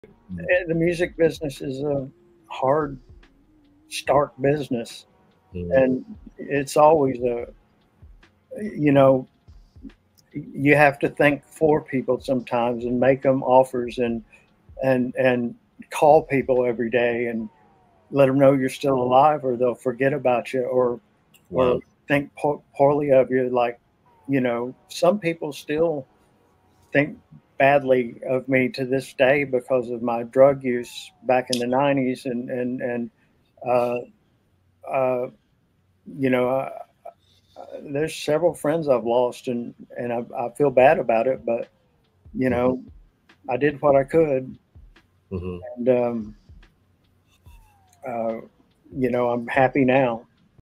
Joey Huffman explains: Surviving the Music Business Overcoming Struggles and Finding Happiness
Joey Huffman explains: Surviving the Music Business Overcoming Struggles and Finding Happiness on Inplosion on BLASTMusic247 YouTube Channel